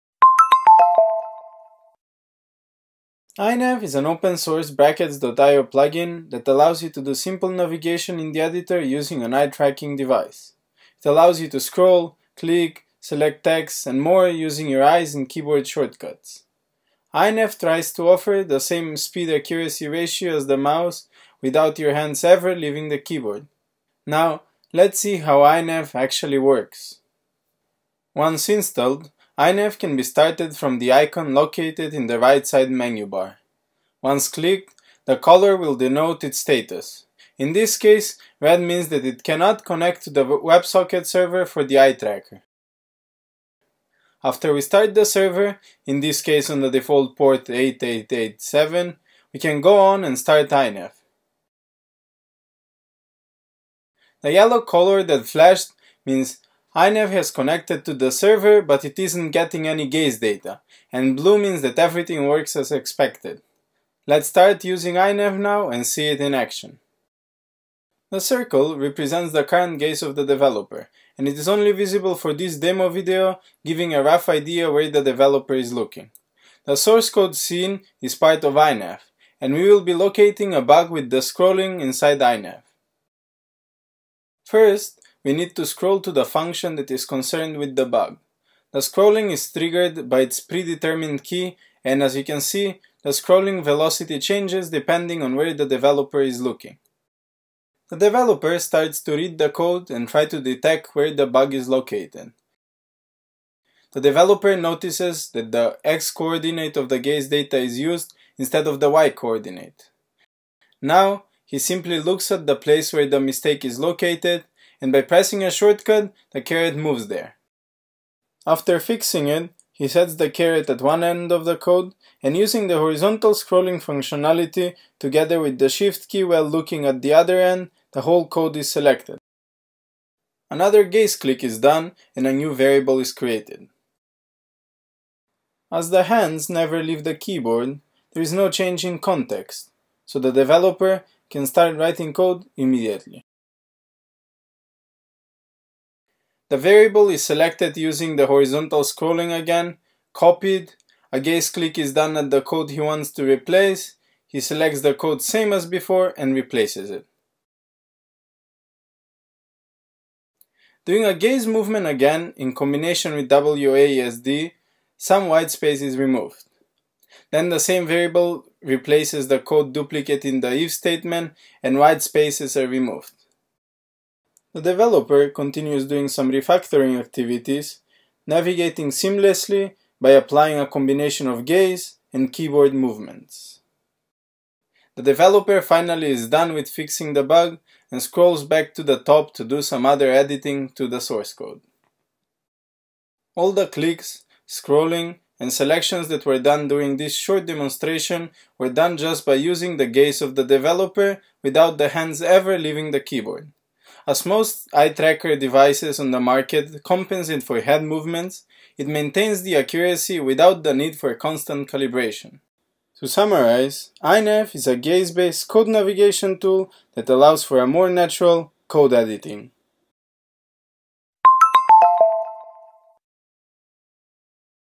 EyeNav: Gaze-Based Code Navigation
EyeNav is a Brackets.io plugin that allows you to do simple navigation in the editor using an eye-tracking device. It allows you to scroll, click, select text, and more using your eyes and keyboard shortcuts.

EyeNav has been developed at Nara Institute of Science and Technology (NAIST), Software Engineering Laboratory.